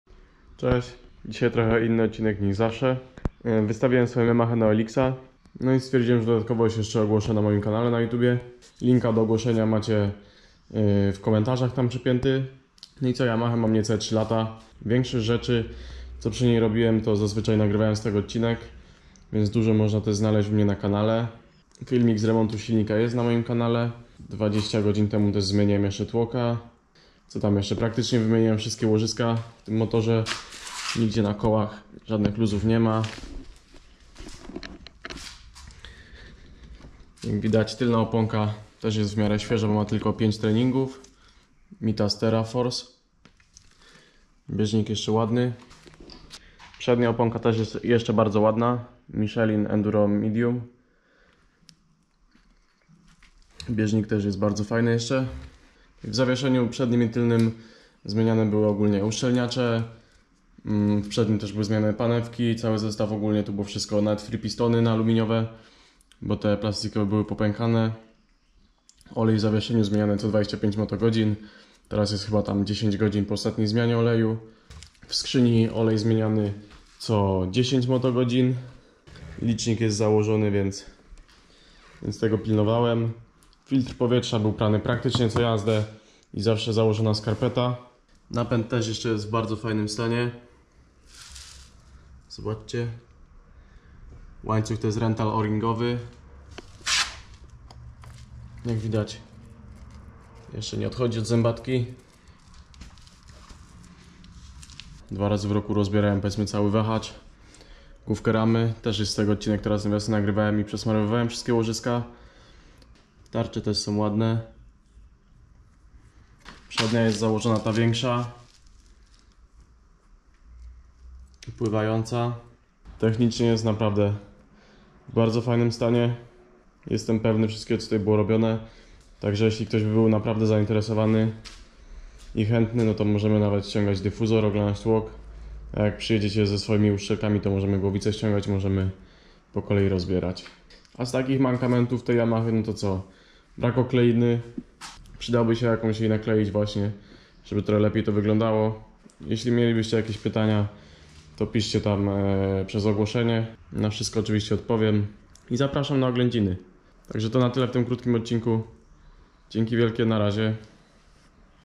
Yamaha YZ125 2014r na sprzedaż !!! (sprzedana)
Krótka prezentacja mojej yamahy, którą wystawiłem na sprzedaż.